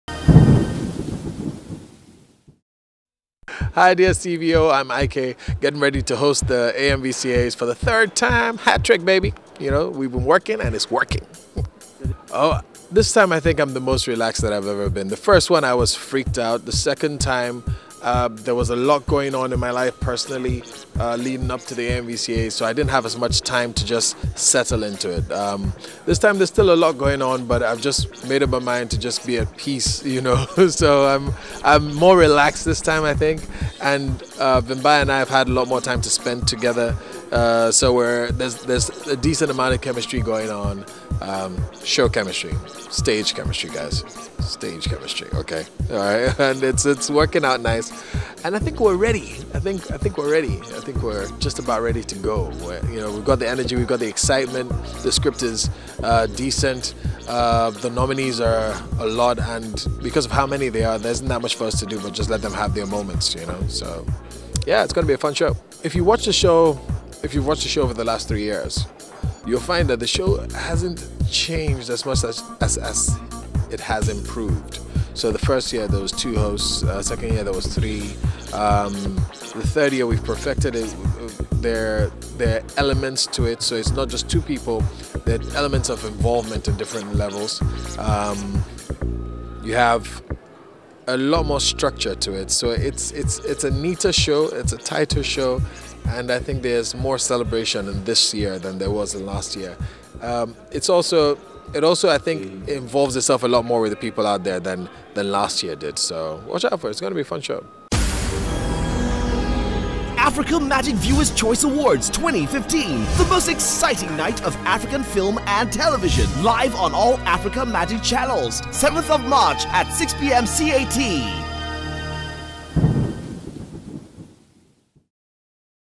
AMVCA2015 host IK ready to bring the magic
IK Osakioduwa is ready to host this year's AMVCA2015 and once again he'll be joined by the lovely, Vimbai Mutinhiri. He'll